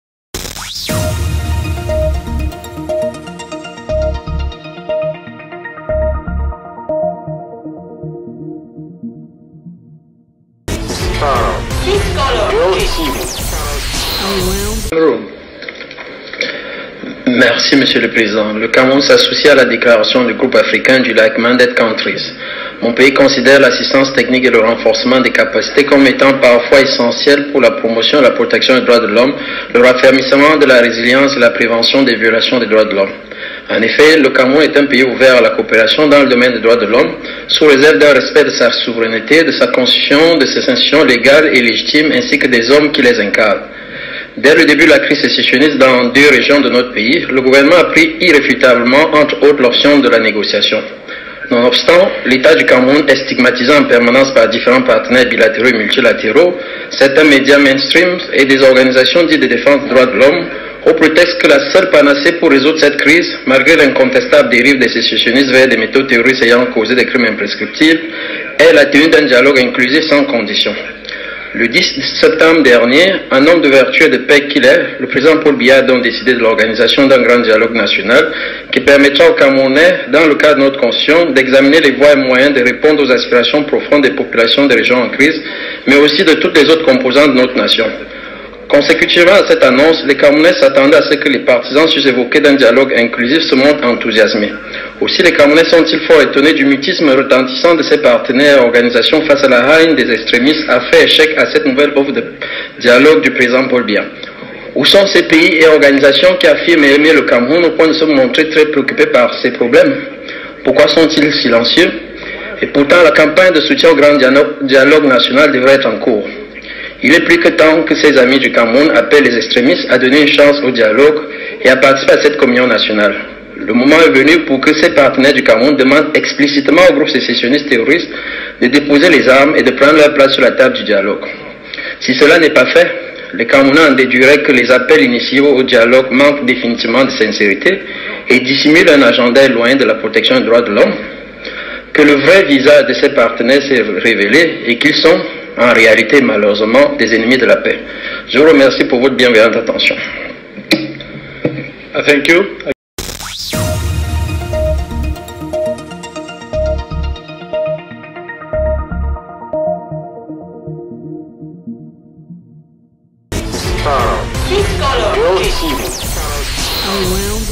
LE GRAND DIALOGUE NATIONAL , LE CHOC DES IDEES INTERVENTION DU CAMEROUN À L'ONU
LE GRAND DIALOGUE A L'ONU : INTERVENTION DU CAMEROUN LORS DE LA DERNIERE SESSION DU CONSEIL
DES DROITS DE L HOMME PAR LE DR CÔME DAMIEN GEORGES AWOUMOU